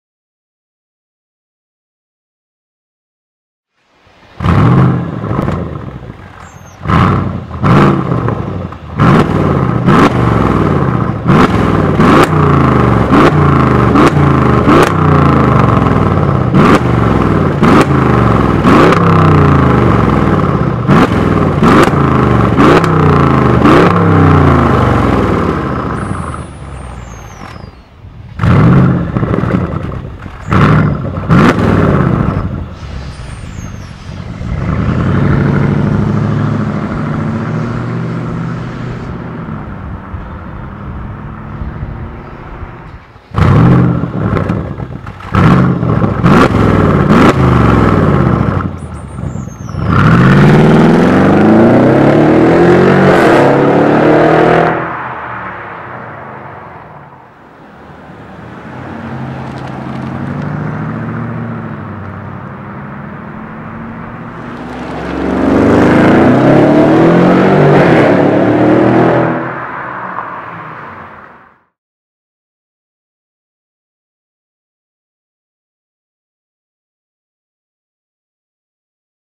MEC Design Mercedes C217 Coupé / A217 Cabriolet S550 / S500 Exhaust - Sound Version Earthquake
MEC Design Exhaust for the Mercedes Benz C217 Coupé / A217 Cabriolet S500 Model. Sound version Earthquake.